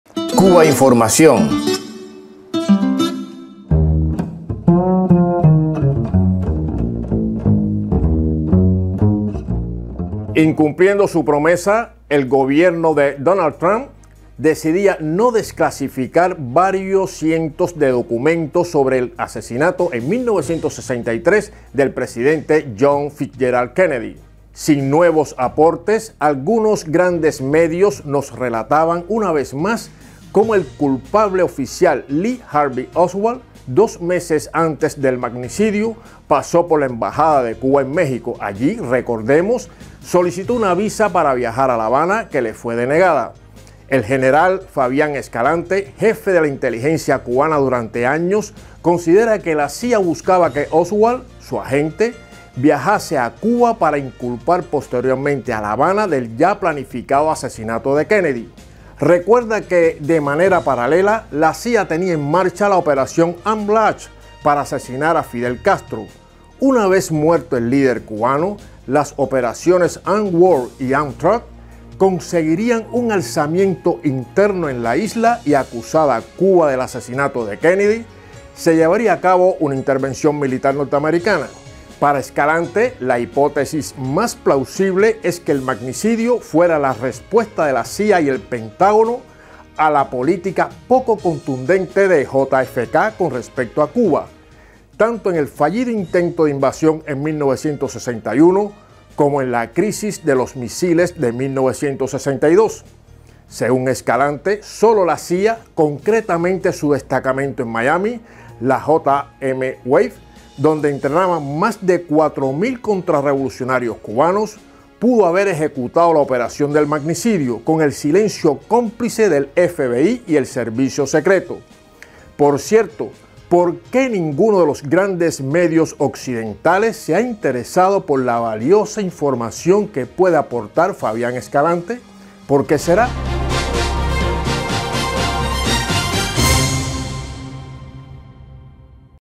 Asesinato de Kennedy: ¿qué opina el exjefe de la Inteligencia cubana?
Cubainformación TV – Basado en un texto de Fabián Escalante - La pupila insomne.- Incumpliendo su promesa, el gobierno de Donald Trump decidía no desclasificar varios cientos de documentos sobre el asesinato, en 1963, del presidente J. F. Kennedy. Edición: Ana Gil. Fuente